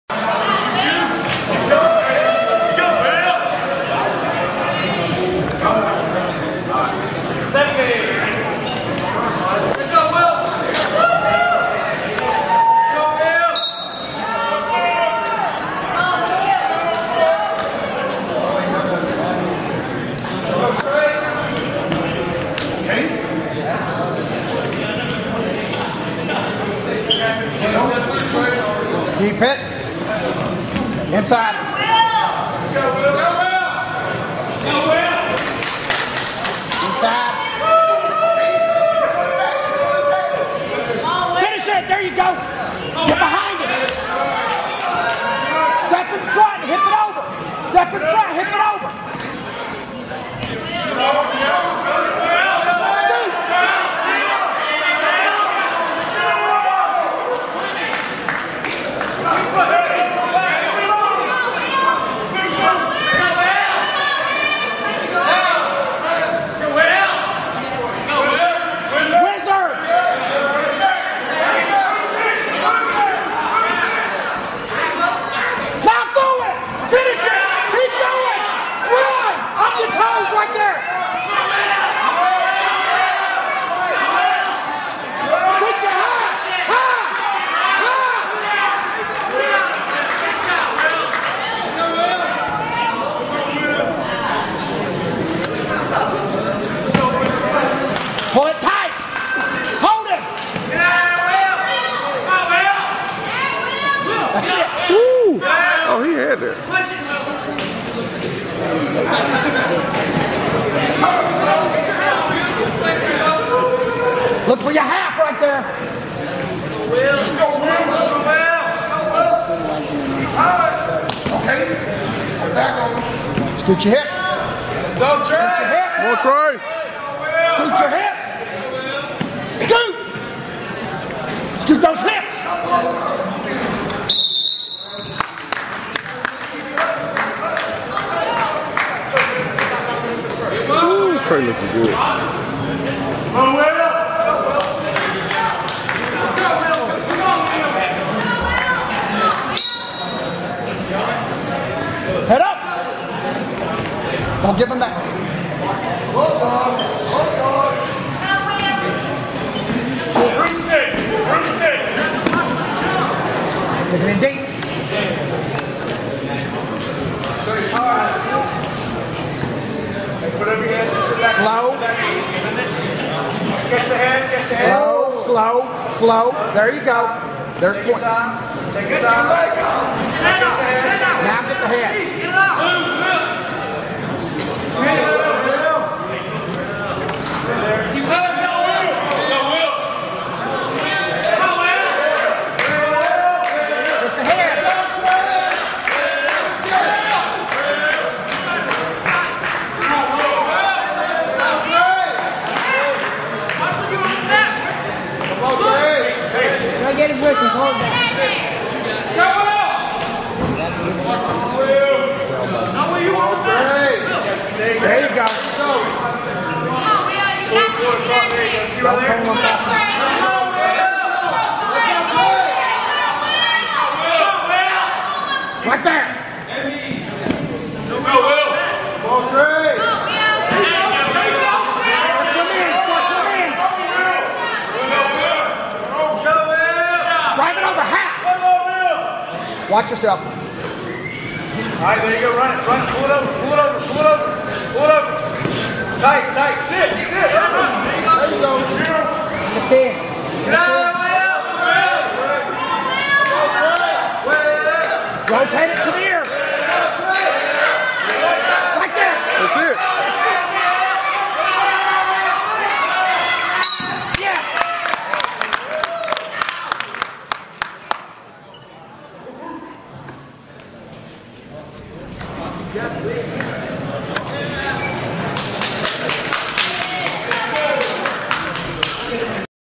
Trey Rhodes Wrestles up 170 Vs Washington Wilkes
Trey has to work hard to pin a 170  from Washington wilkes.